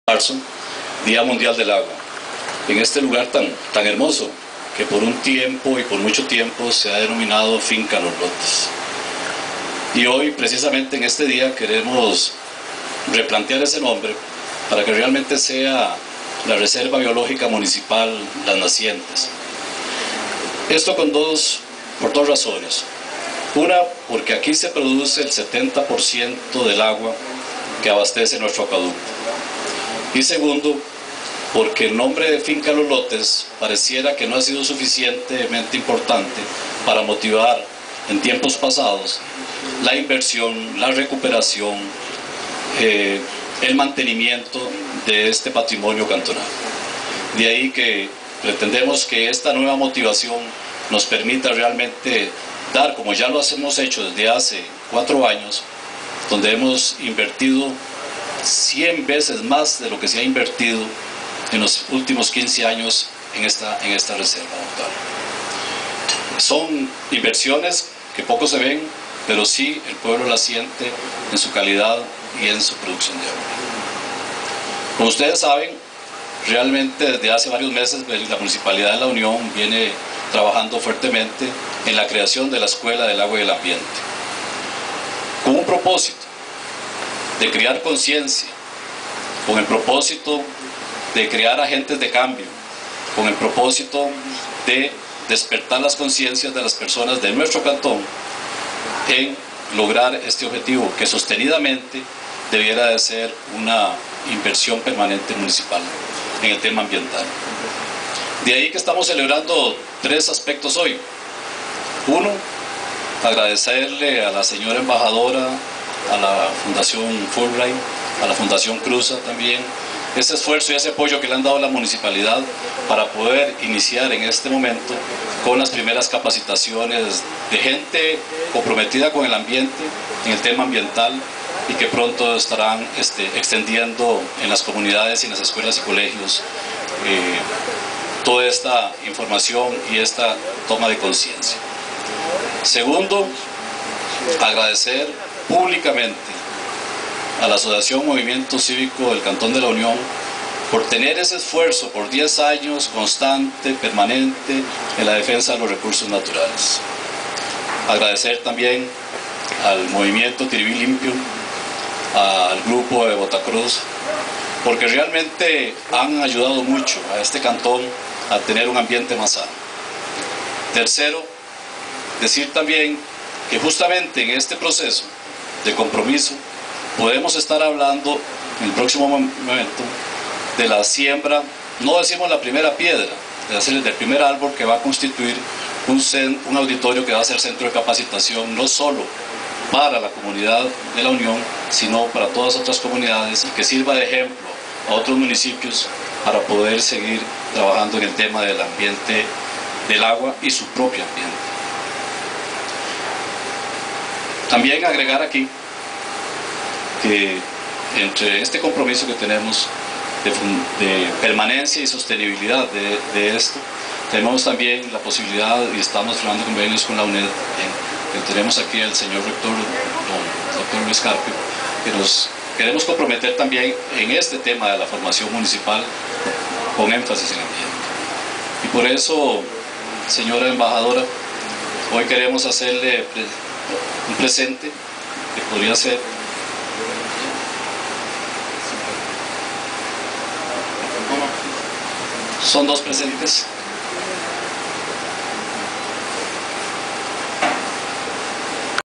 DIA DEL AGUA en La Unión
Esta mañana se celebró el Día Internacional del Agua en la finca Los Lotes propiedad de la municipalidad de La Unión. A la actividad asistieron la embajadora de los Estados Unidos Anne Andrew, los diputados Martín Monestel e Ileana Brenes, miembros del concejo municipal e invitados especiales.

Durante el evento se plantó un árbol como inició a lo que será la construcción de la Escuela del Agua y del Ambiente y un anfiteatro para las charlas ambientales  "sin paredes" que se realizarán en el lugar.

Participaron además miembros de la Fundación Fulbright, Movimientos Tiribí LImpio y Cívico.

Personeros de la Escuela del Agua y Ambiente entregaron un reconocimiento a tres agrupaciones locales MOCICU, Tiribí Limpio y Grupo Botatour por su aporte y trabajo en temas medioambientales.

El alcalde Julio Rojas se refirió a la celebración